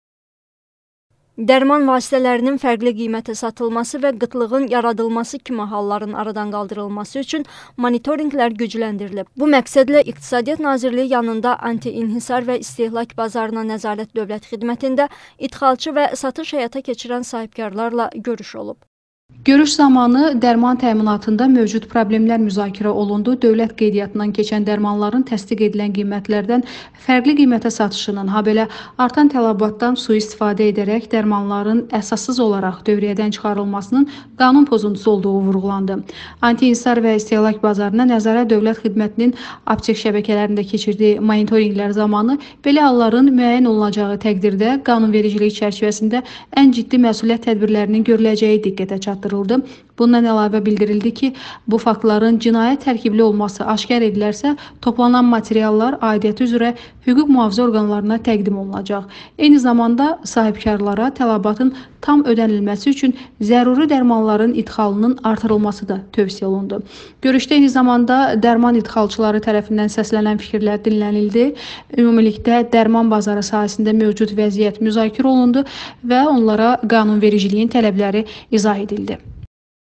Dərman idxalçıları ilə görüş olub
Dərman qıtlığı və qiymət artımı ilə bağlı müzakirə aparılıb